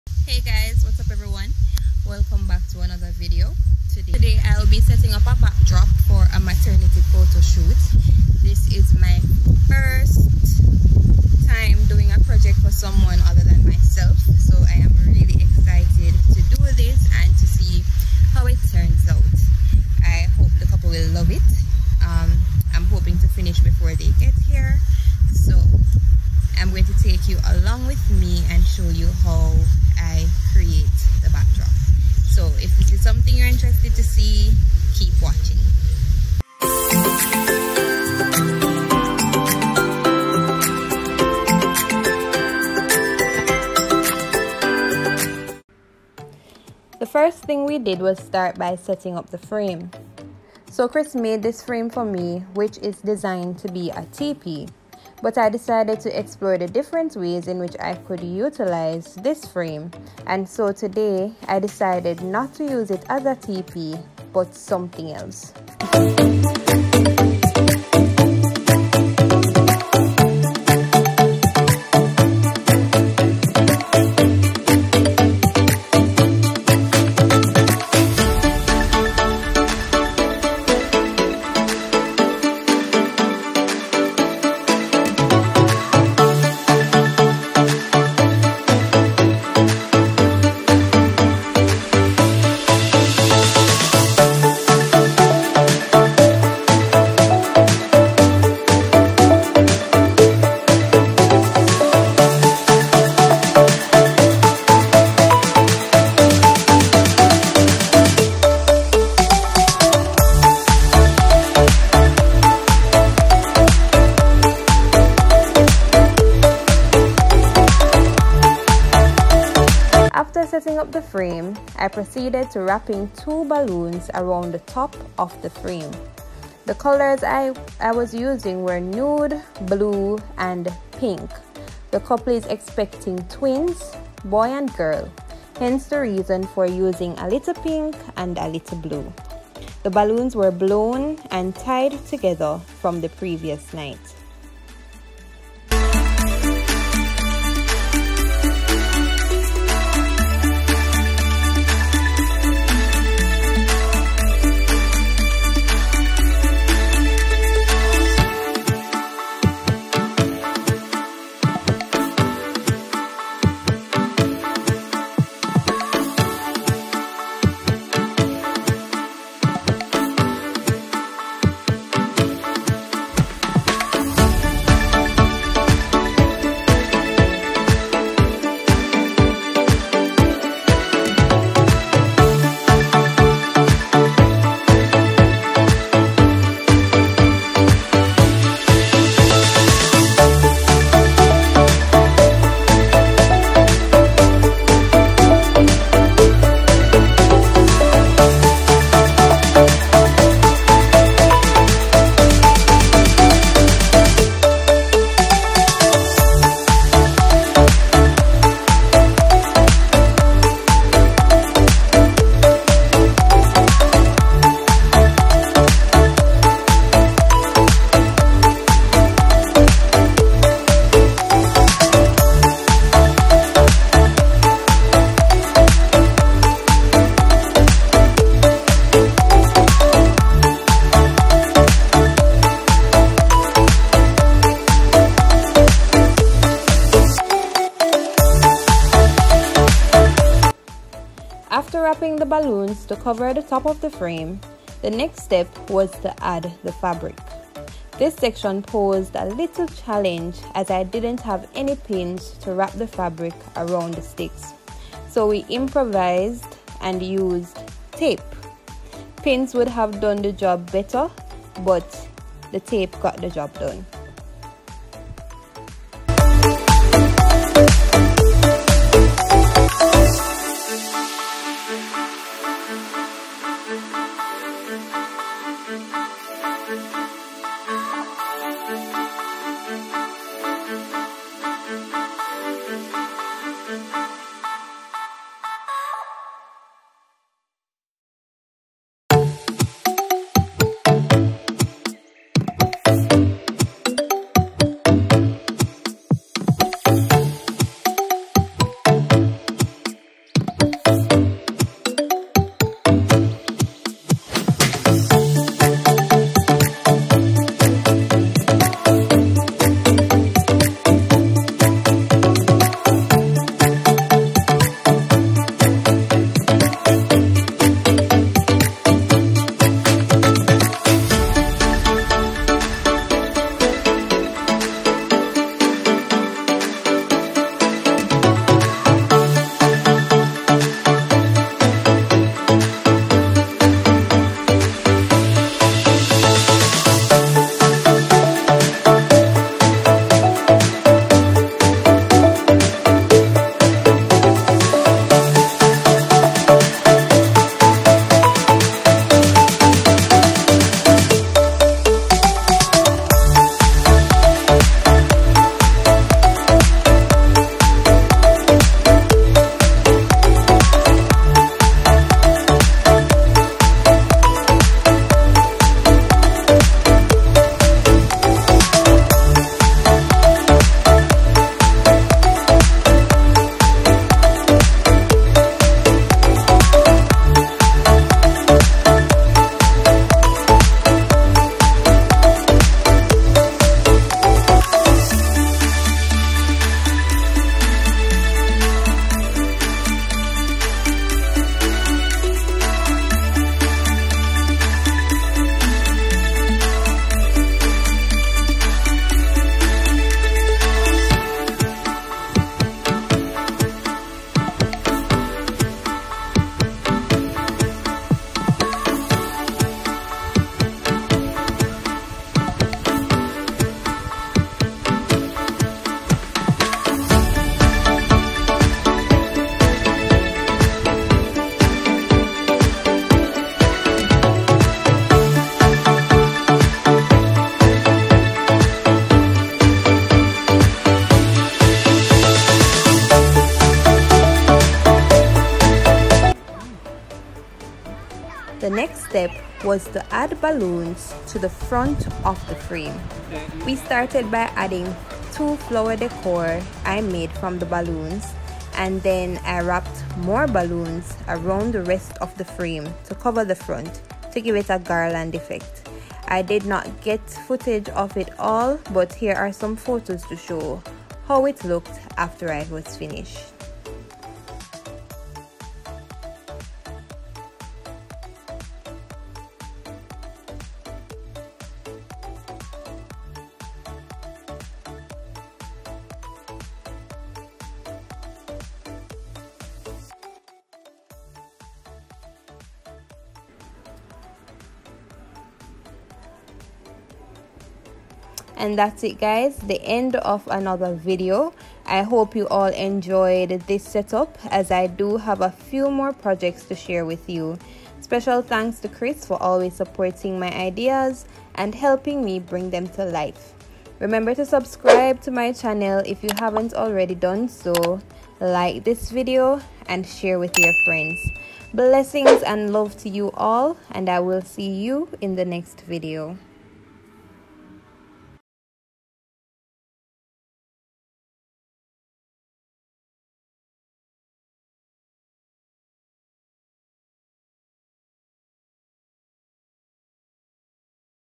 BALLOON BACKDROP SETUP||LATTIE'S CORNER
Hey Guys!
In today's video, I did a backdrop setup using balloons for a maternity photoshoot. I hope you will enjoy watching.

Items used in video:
Balloons (pink)
Balloon Pump
Balloon sizer

S U B S C R I B E S U B S C R I B E!